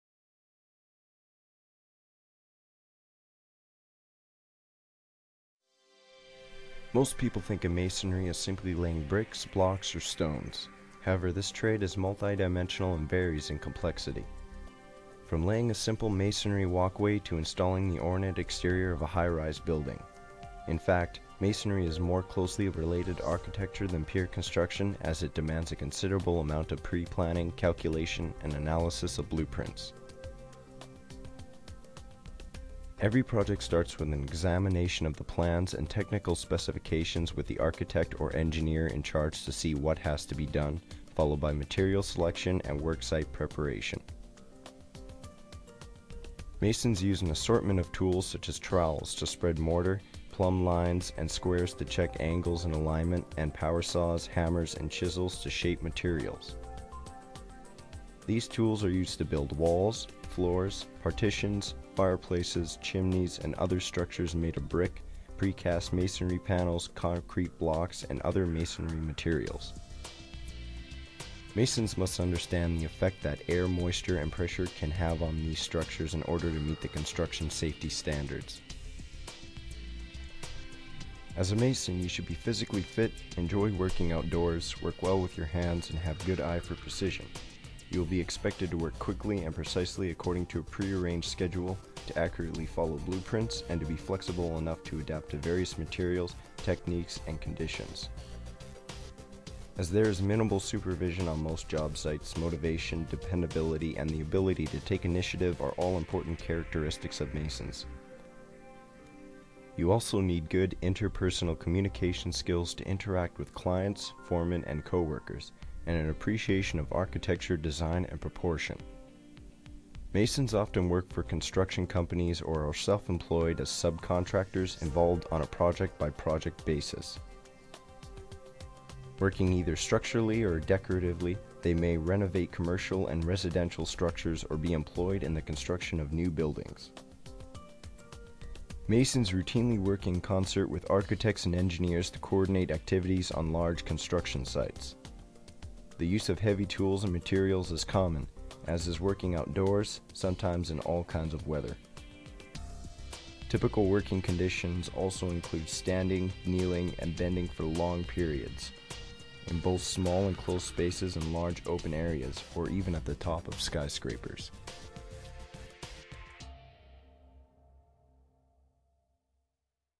Skills Canada 2008 - Brick Masonry
KCDC presents the Skills Canada Competition videos. Each video is a demonstration of the trade in action during the Skills Canada Competition 2008, which was hosted at Stampede Park in Calgary, Alberta Canada.